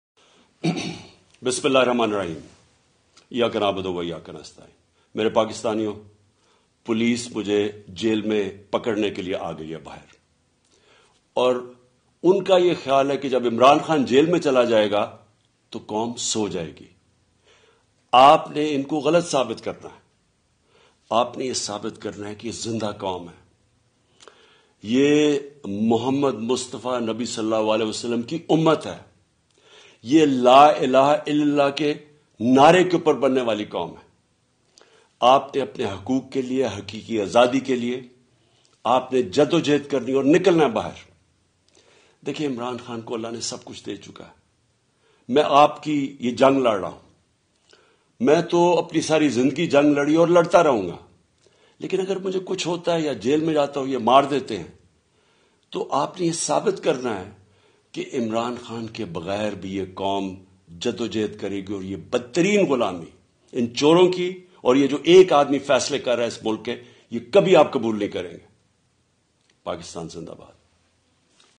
message to the nation to stand resolute and fight for haqeeqi azadi and rule of law.
Imran khan message to nation.